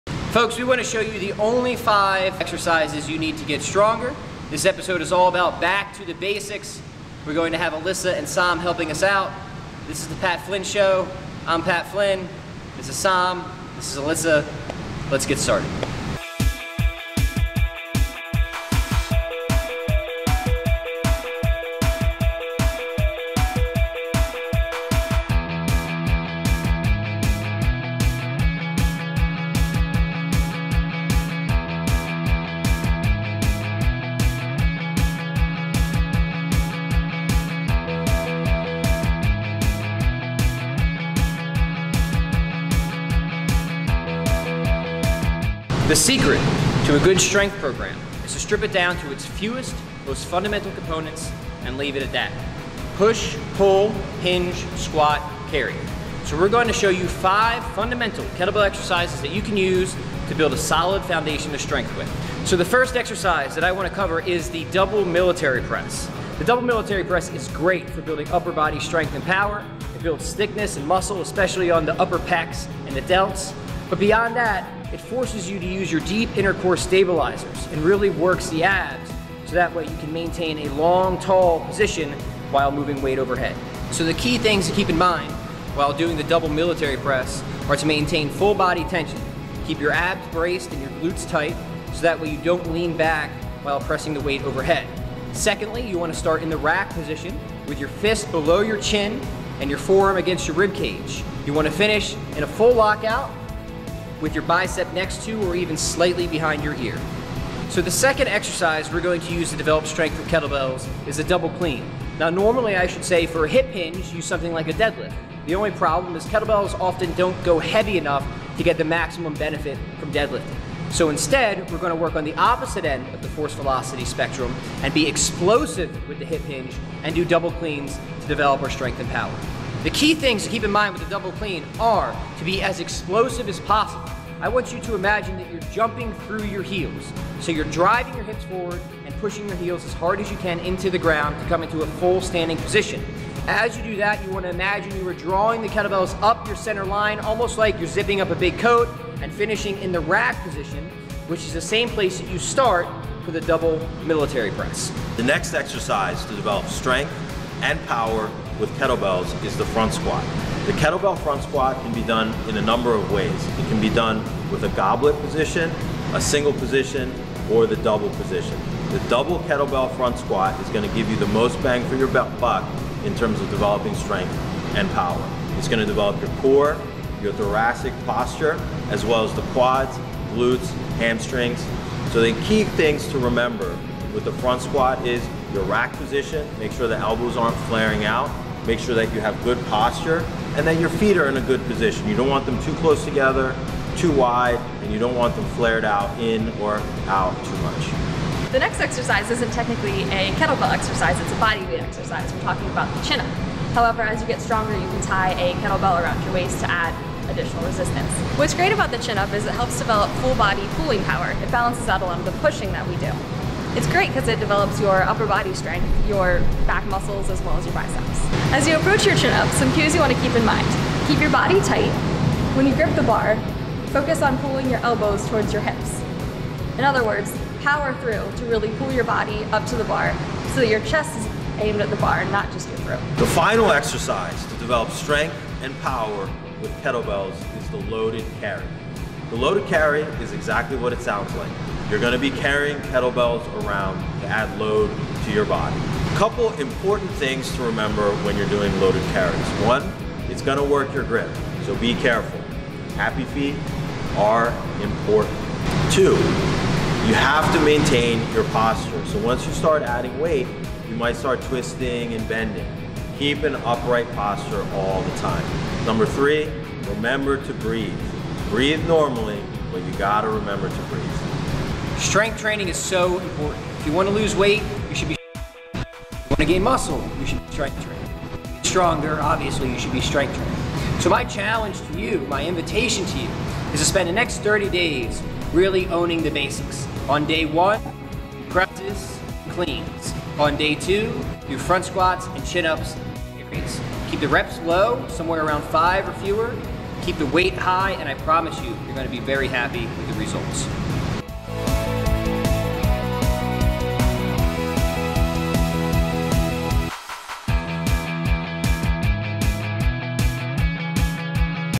The Only 5 Exercises You Need to Get Stronger with Kettlebells
Wanted to get back to basics and discuss the 5 fundamental moves you can use to start developing strength and power with kettlebells.